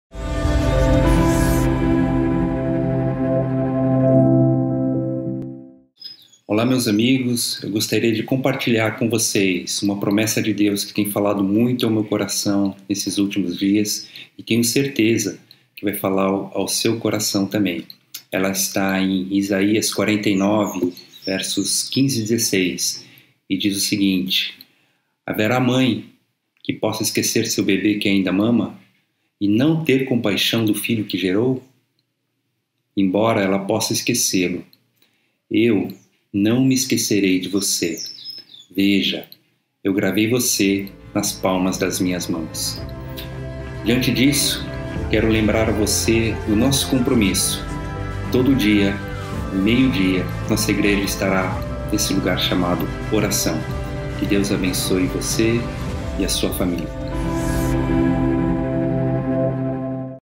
Experimente confiar nas promessas de Deus | Yuji Marini
Esta é a segunda mensagem devocional desta série "Experimente confiar nas promessas de Deus".

Que esta promessa da Palavra de Deus, compartilhada pelo presbítero Yuji Marini sobre o texto da Palavra de Deus em Isaías 49.15-16, fale profundamente ao seu coração.